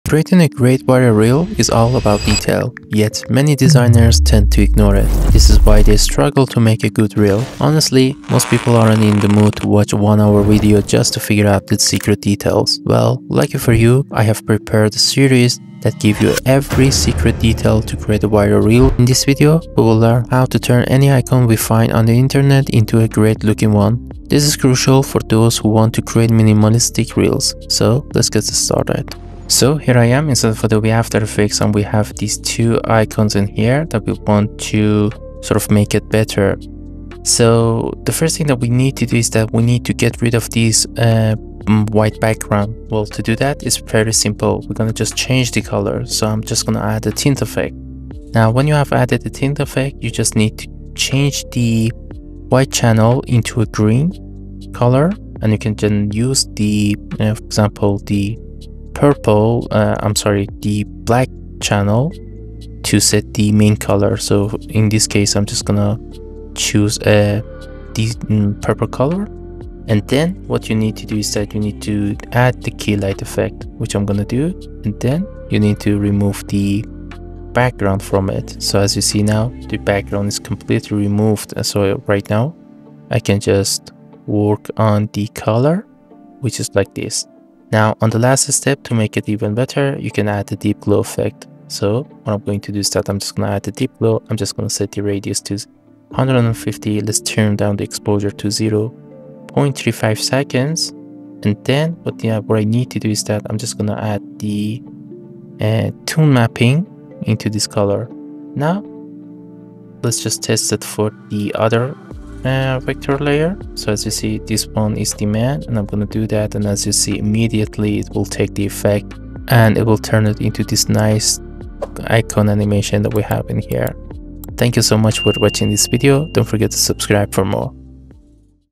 Turn Any internet Icon Into breathtaking animations | Viral Reel Secret Ep:1
👋 Hello and welcome to this video. In this video, I am going to show you how you can create reels Cordia in Adobe After Effects .

🛠 Tools Used:

Deep Glow

📄Chapters:

00:00 Intro
00:27 Secret